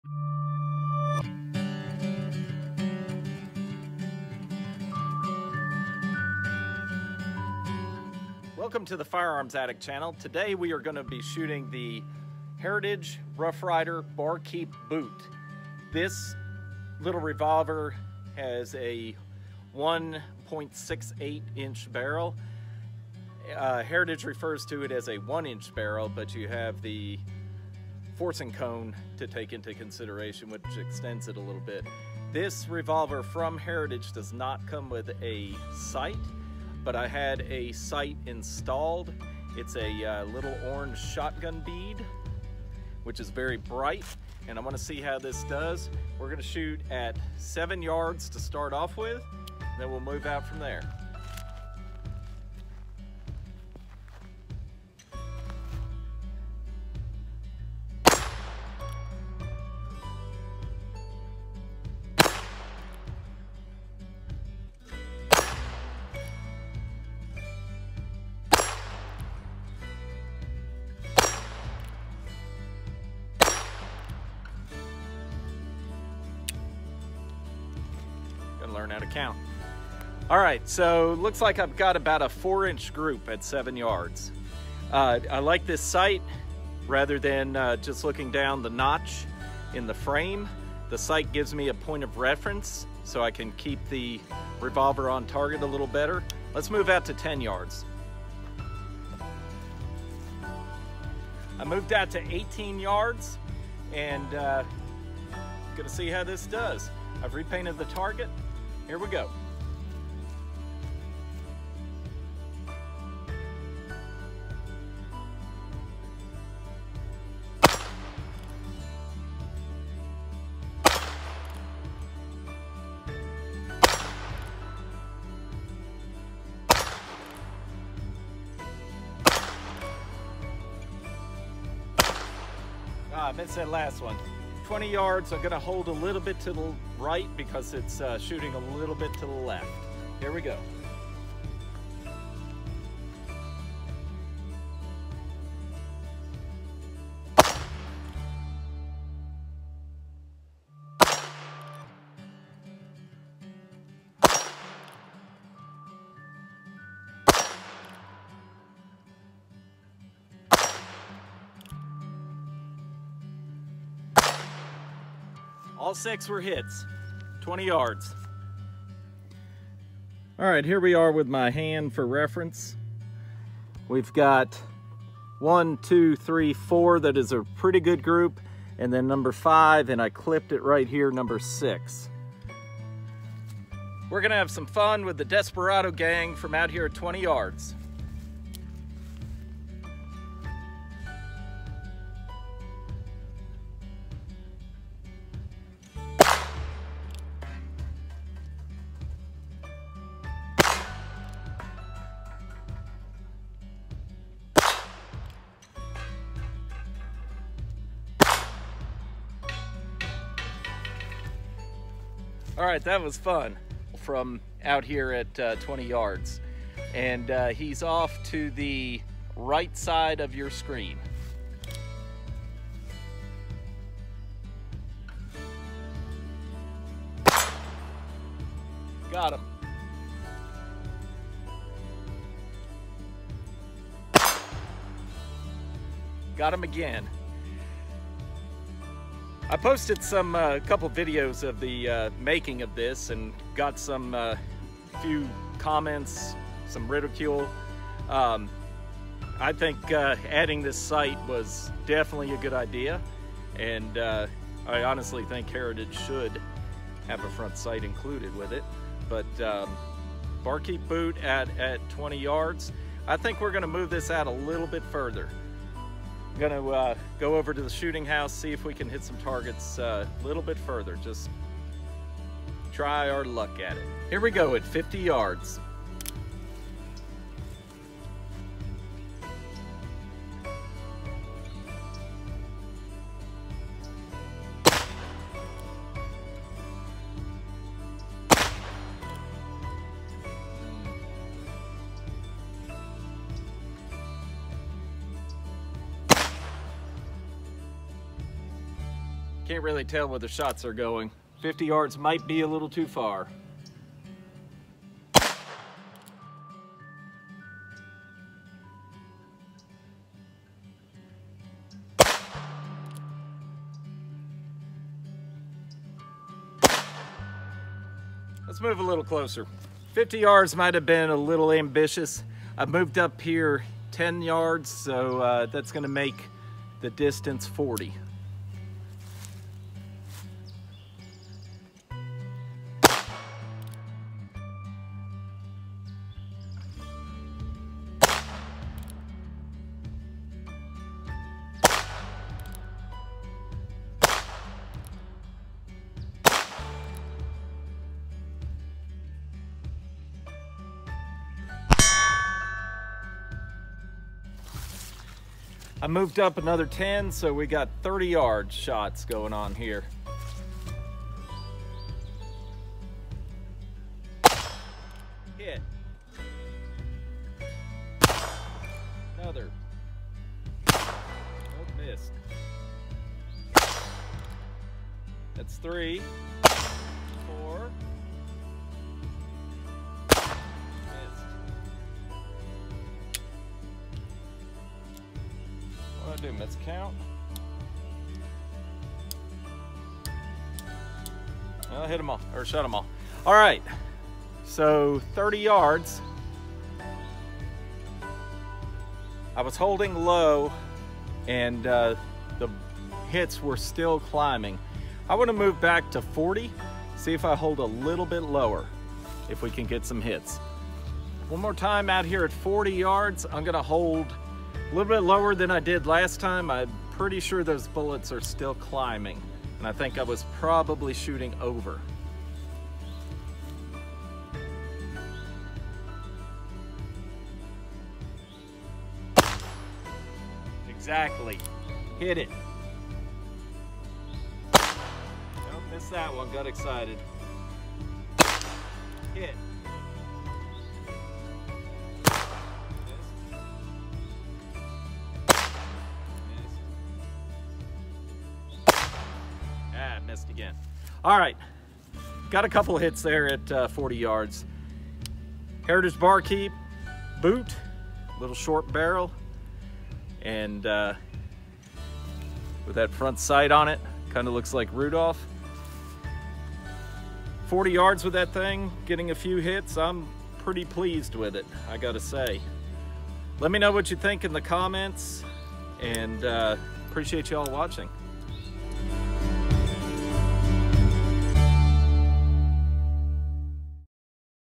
Heritage Barkeep Boot, how far will it shoot?
In this video we shoot the Heritage Barkeep Boot, to see how far will it shoot.